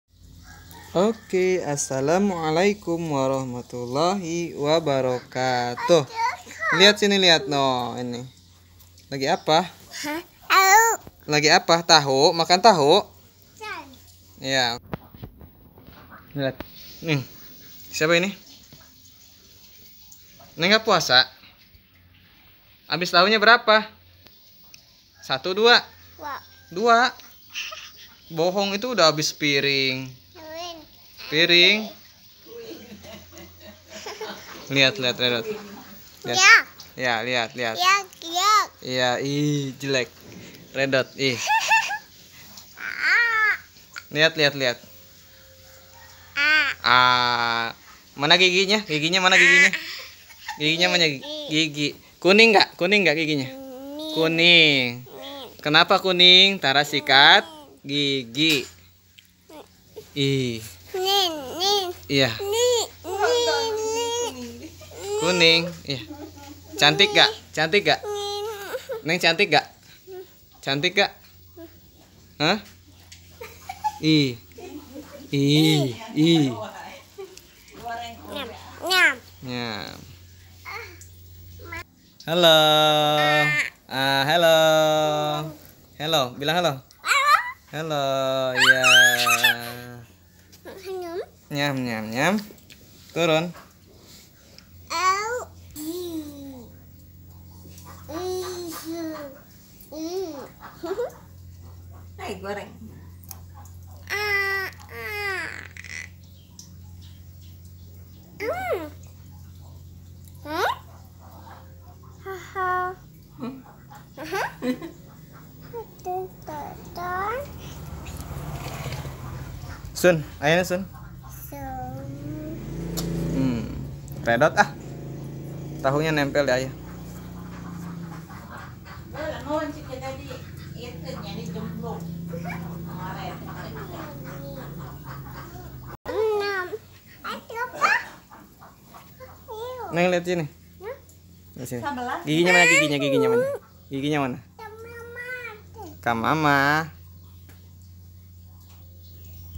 Belepotan Pamiarsa
Fathiyaturahma Tazania Putri belum Bisa sikat gigi ampun dah karena masih 2th, sukanya makan tahu Sumedang tapi yang dimakan hanya bagian dalamnya saja yang putih nya . kulitnya dibuang dan dilempar.